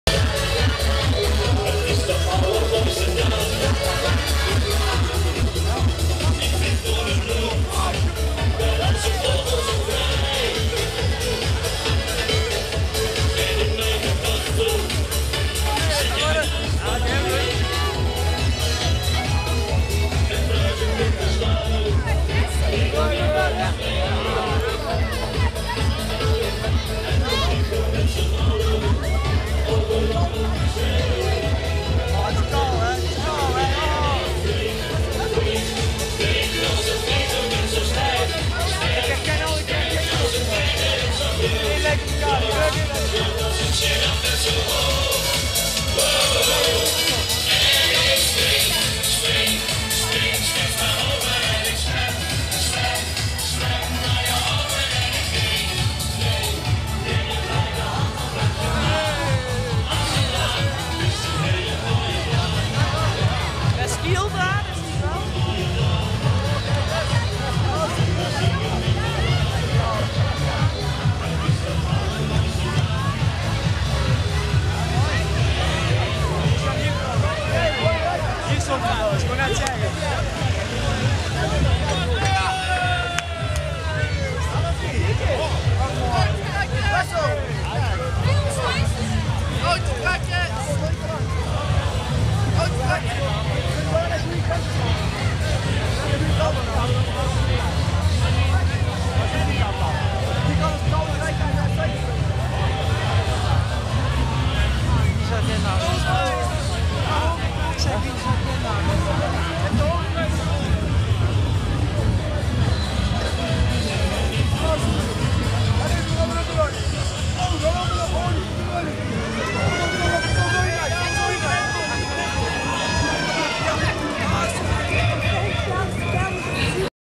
Carnavalswagen 'T Taphûkske 2010
Carnavalswagen van  'T Taphûkske 2010 in Handel. 3e Juryprijs, 3e Publieksprijs